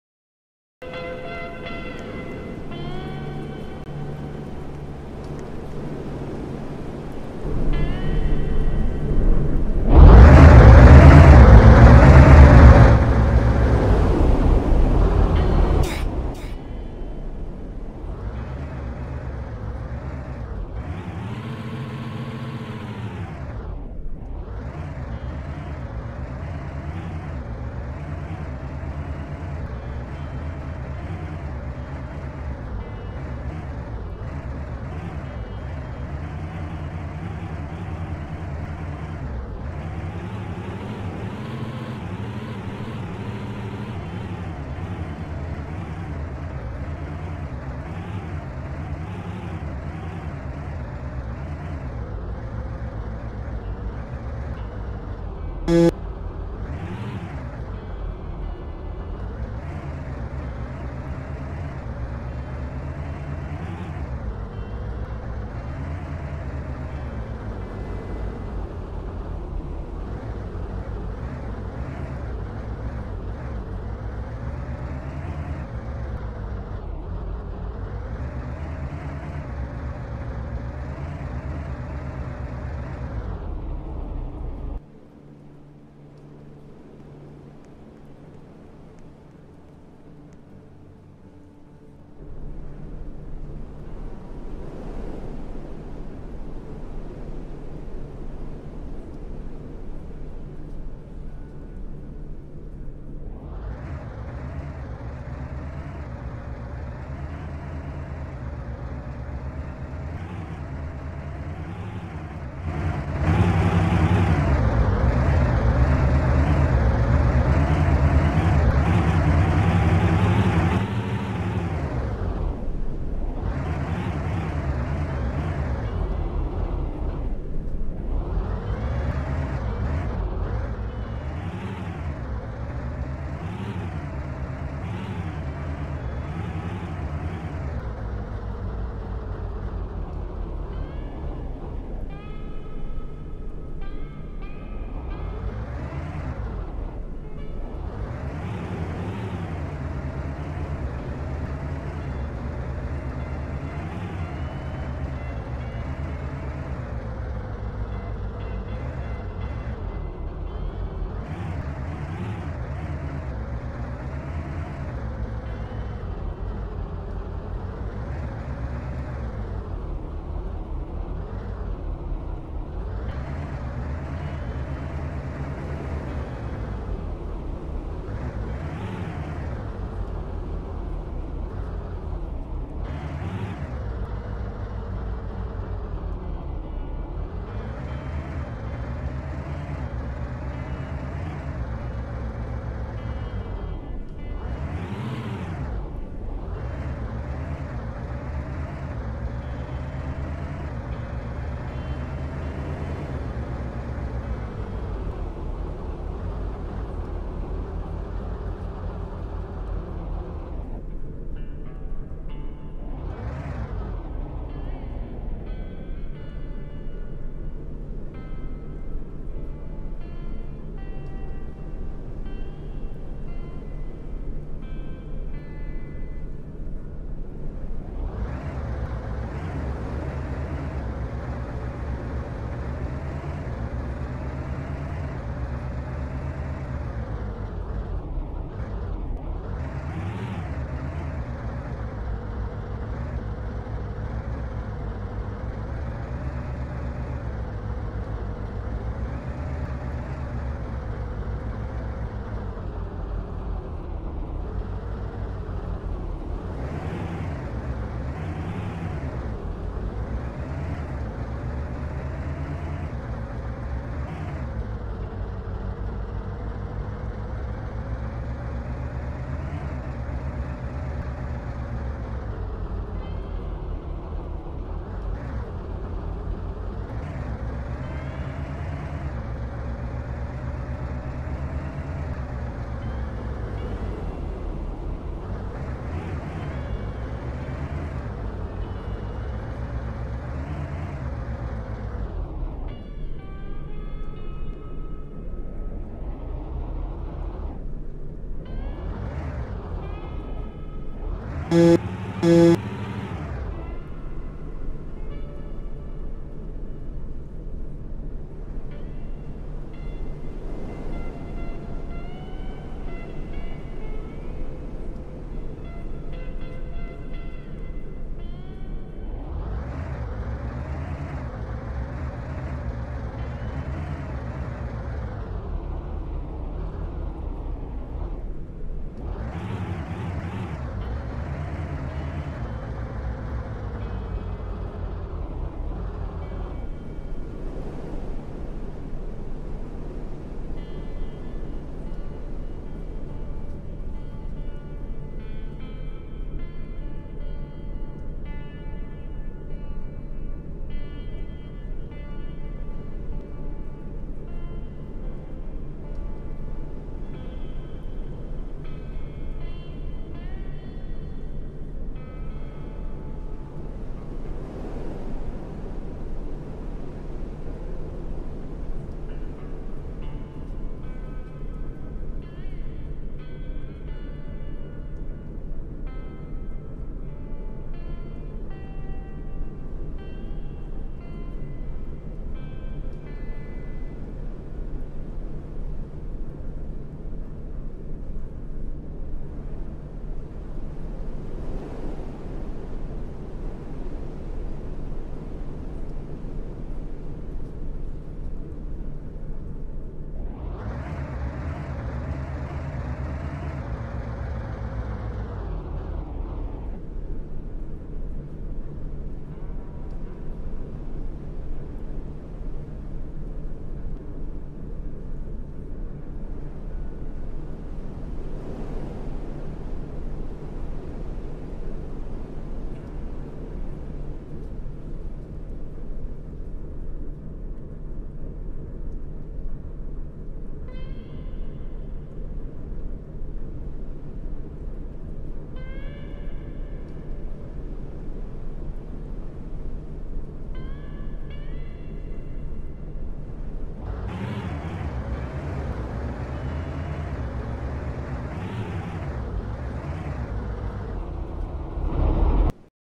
Truck game that I quit halfway through because I'm not doing that completely maybe another time aaaa
Longest video name ever.